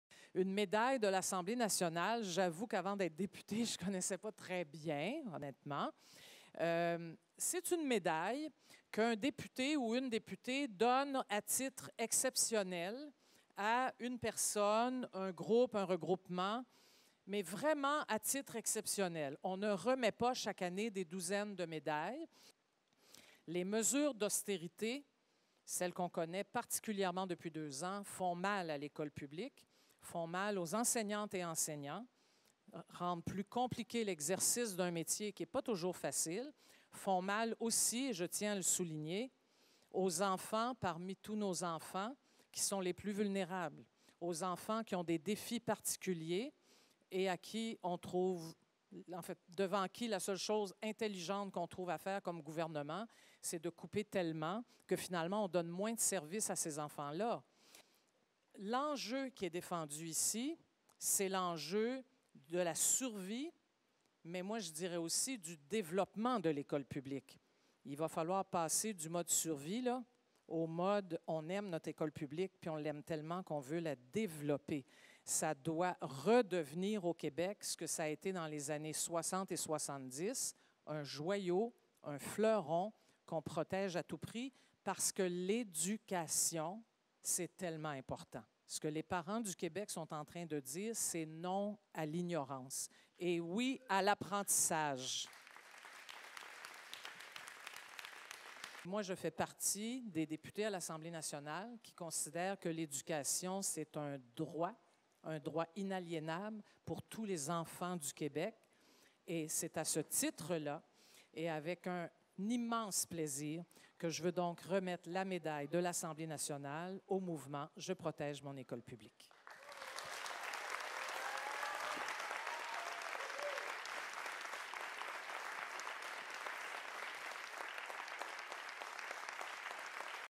Médaille de l’Assemblée nationale au mouvement Je protège mon école publique
Françoise David, députée de Gouin et porte-parole de Québec solidaire, a tenu à souligner la combativité des parents du mouvement Je protège mon école publique. Ce mouvement a pris naissance dans la circonscription de Gouin puis est devenu un mouvement national grâce aux efforts de milliers de parents à travers le Québec. Mme David et plus d’une centaine de parents réunis à l’école Saint-Jean-de-Brébeuf dans le quartier Rosemont-La Petite-Patrie à Montréal ont tenu à lancer un message au gouvernement québécois à la veille du dépôt du budget : les parents qui luttent depuis près d’un an pour la sauvegarde de l’école publique ne se contenteront pas de vagues promesses de réinvestissements. L’argent doit être au rendez-vous pour les écoles publiques du Québec.